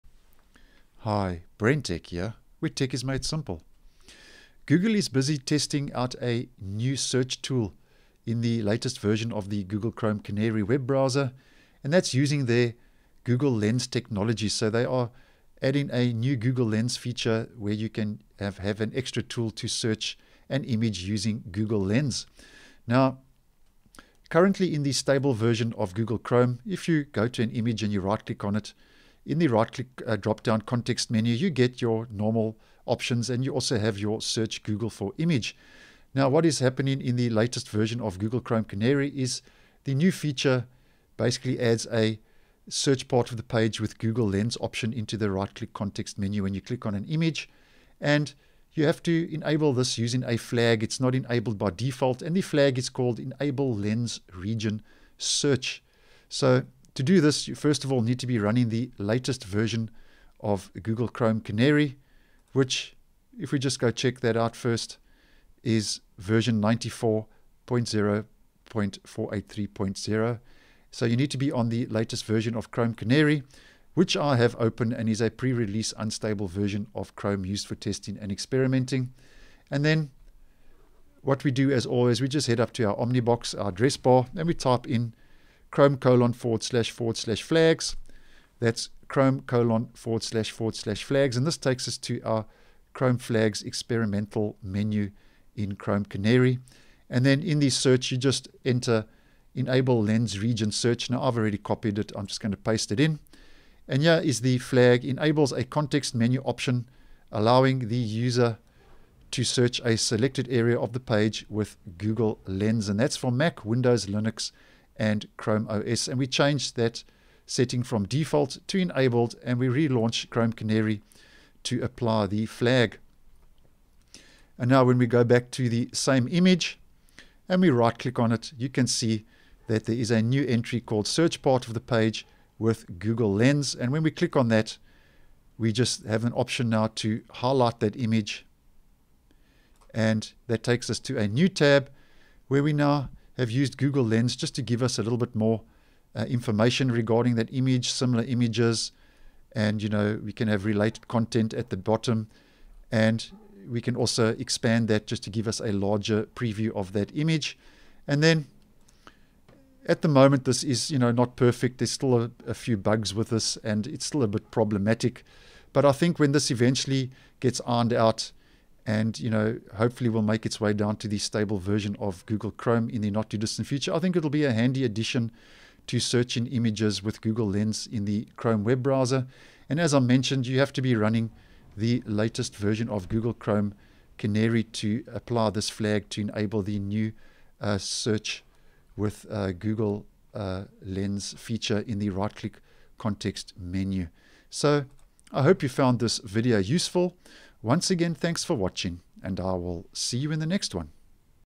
Google is testing a new search tool in Chrome using Google Lens
This feature is available in Chrome Canary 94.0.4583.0.